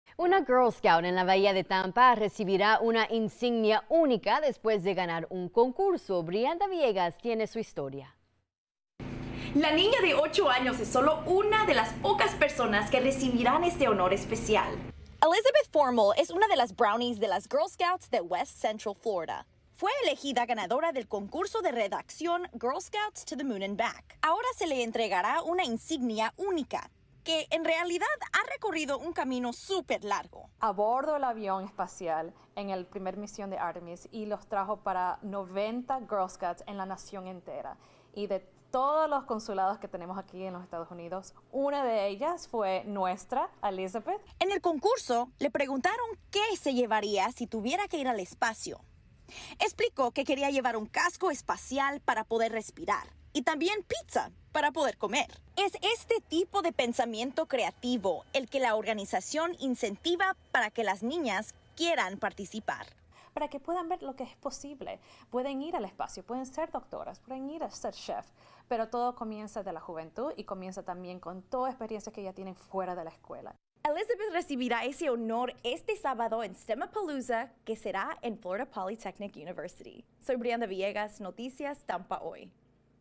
Niña scout de la Bahía de Tampa recibe una insignia especial
Una Girl Scout, de 8 años, recibirá una insignia única que ha estado, literalmente, ¡fuera de este mundo!